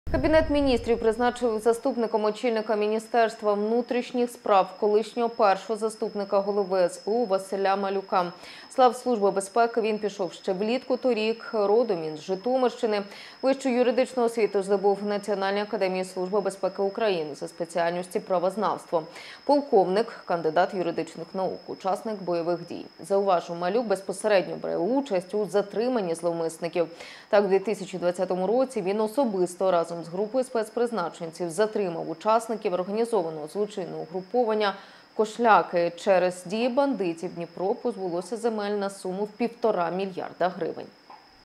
Новий заступник міністра внутрішніх справ
Уряд призначив заступником очільника Міністерства внутрішніх справ колишнього першого заступника голови СБУ Василя Малюка. З лав Служби безпеки він пішов ще влітку торік.  Родом він з Житомирщини.  Вищу юридичну освіту здобув у Національній академії Служби безпеки України за спеціальністю правознавство. Полковник, кандидат юридичних наук. Учасник бойових дій. Малюк безпосередньо бере участь у затриманні зловмисників. Так, в 2020 році він особисто разом з групою спецпризначенців  затримав учасників організованого злочинного угруповання "Кошляки". Через дії бандитів Дніпро позбулося земель на суму в 1,5 млрд грн.